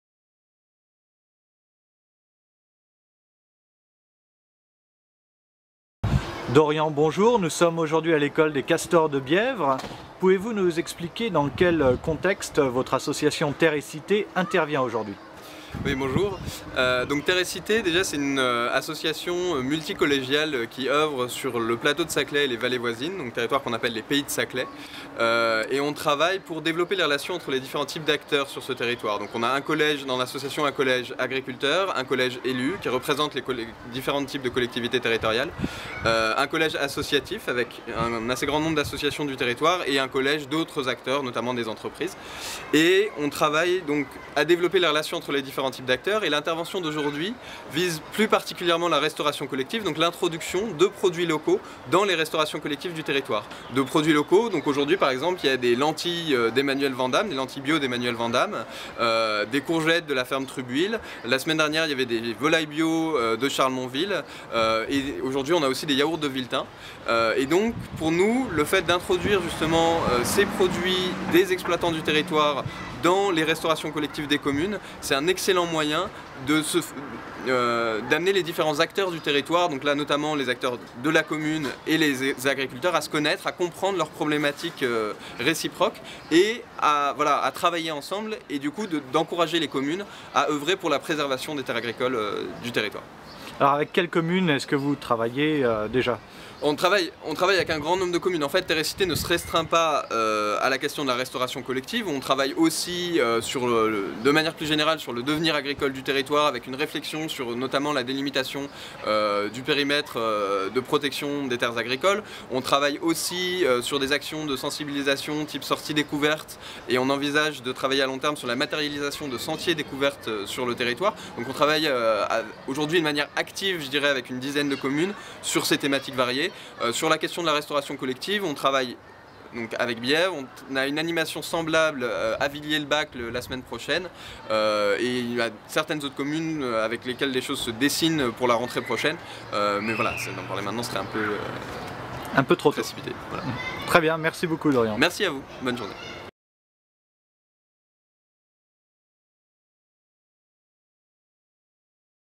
Dorian Spaak de Terre et Cite
La commune de Bièvres a engagé depuis plusieurs années sa restauration collective dans une démarche qualité exemplaire, en travaillant avec des produits bio et de proximité, et en faisant redécouvrir aux enfants des produits et recettes peu communes. Les produits sont commandés et travaillés sur place par l'équipe de restauration de la commune.

Elle a organisé Vendredi 17 Juin un repas de produits locaux en partenariat avec l'association Terre et Cité, qui oeuvre pour le maintien des terres agricoles sur le Plateau de Saclay et le développement de filières alimentaires de proximité. Cet évènement nous permet de faire connaître la mise en place d'un approvisionnement régulier de produits de proximité, et la relation naissante et renforcée avec les producteurs du Plateau de Saclay.

Les produits des exploitations agricoles du territoire seront alors expliqués aux enfants par le biais de jeux et d'animations diverses.

Cette initiative pionnière d'introduction de produits locaux ouvre des perspectives intéressantes pour les autres communes des Pays de Saclay.